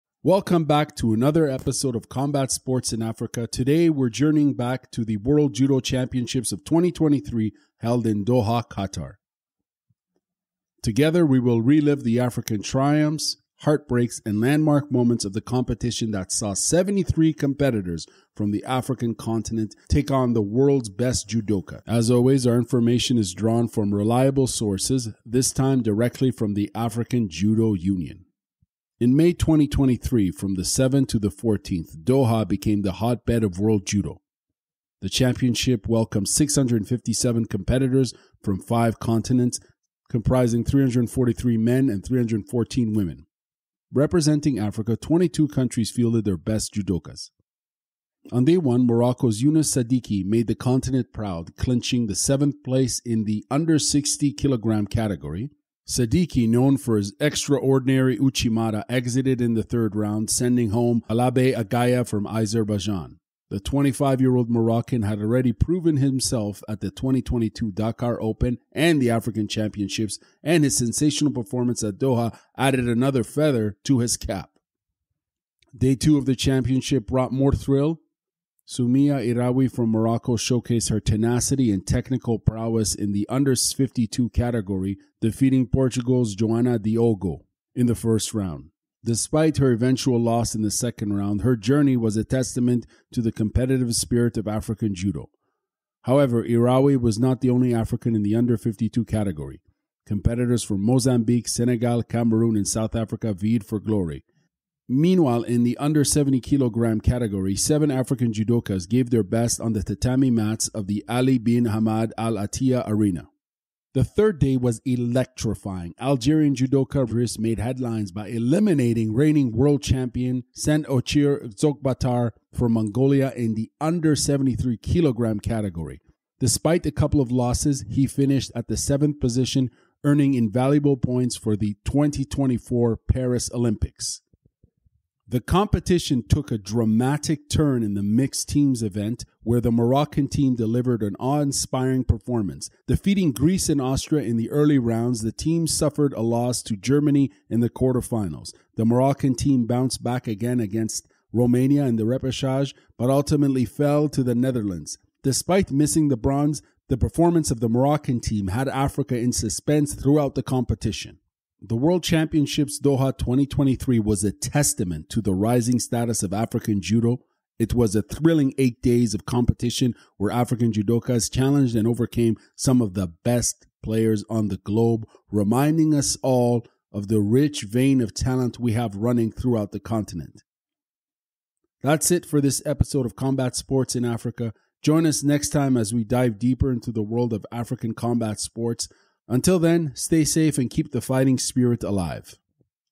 Gripping Action: African Judokas at the World Judo Championships 2023
Judo Unleashed: Journey with us into the electrifying arena of the Baku 2023 World Judo Championships. From Younes Saddiki's compelling performance to Soumiya Iraoui's stunning passes, and Dris's incredible feat of defeating the reigning Mongolian World Champion Tsend Ochir, we've got it all covered! Tap in for the exhilarating details on how Morocco finished fifth in the mixed team's event! For more in-depth information, refer to our sources:

• "World Judo Championships 2023 Day 1: Morocco's Younes Saddiki takes 7th place in the 60kg category" - Check it out
• "WJC 2023 Day 3: Algeria's Dris creates the feat by eliminating reigning Mongolian World Champion Tsend Ochir in the 73kg category" - Read
• "World Judo Championships Doha 2023: Mixed teams - stunning Morocco finished fifth" - Get the details